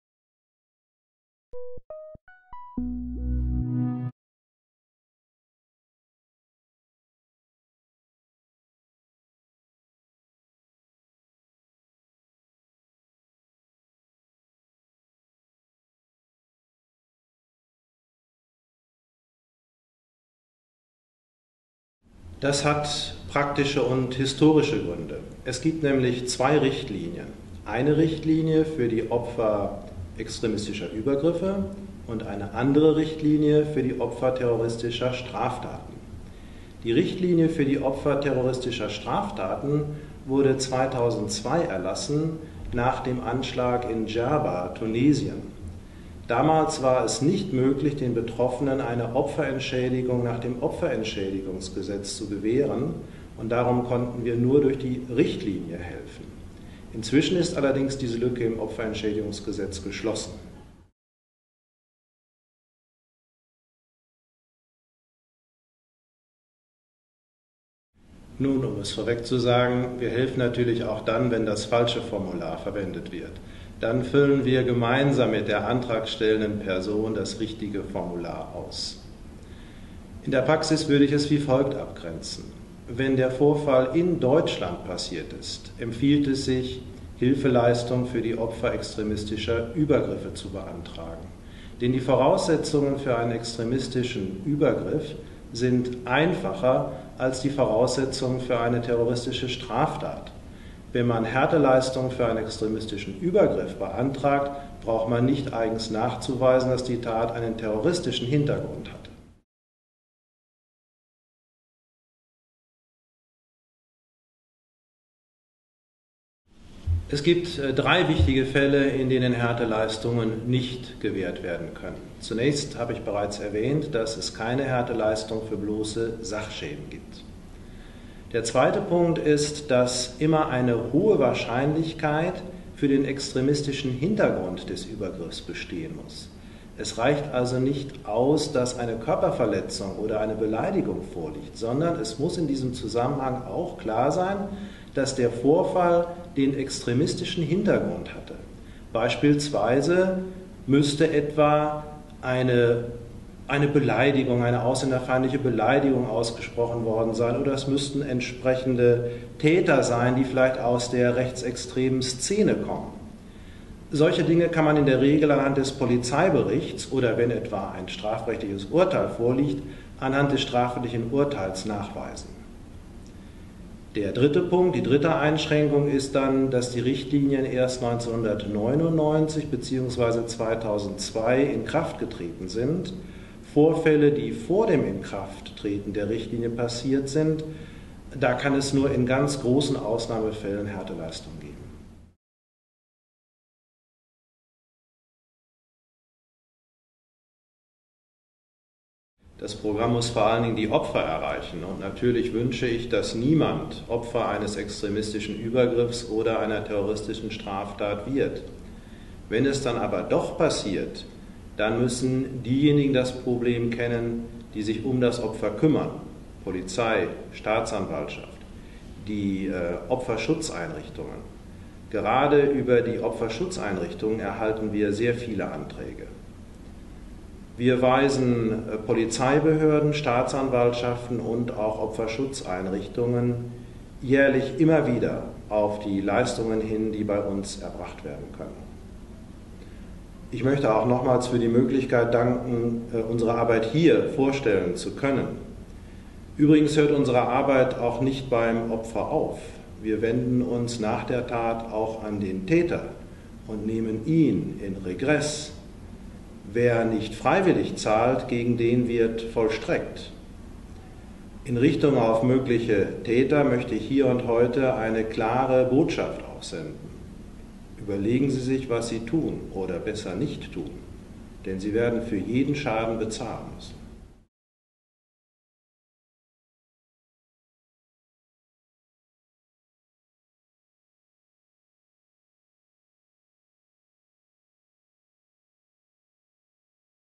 Opferhilfe - Interview mit Herrn Friehe, dem Präsidenten des Bundesamt für Justiz (Teil 2)
Es zählt zu den zentralen Aufgaben des Staates, die Freiheit des Einzelnen vor Extremismen jeder Art zu schützen und zu verteidigen. Der Deutsche Bundestag stellt aus diesem Grunde jährlich Finanzmittel bereit, mit denen Opfern extremistischer Übergriffe eine Härteleistung als Soforthilfe zugesprochen werden kann. Die Härteleistungen sind eine Geste der Solidarität, die unsere Gesellschaft mit den Opfern verbindet; zugleich bringen sie die gesellschaftliche Ächtung zum Ausdruck, der die verübten Taten begegnen. Auch Opfer von terroristischen Straftaten können Härteleistungen erhalten; deren Gewährung ist Teil der vielfältigen Maßnahmen zur Bekämpfung, Ächtung und Verhinderung solcher Taten. Zuständig für die Entscheidung über die Gewährung von Härteleistungen ist das Bundesamt für Justiz. Mehr zu diesem wichtigen Thema erfahren Sie auf der Internetseite Dort finden Sie auch die vom Bundesamt für Justiz bereitgestellten Antragsformulare.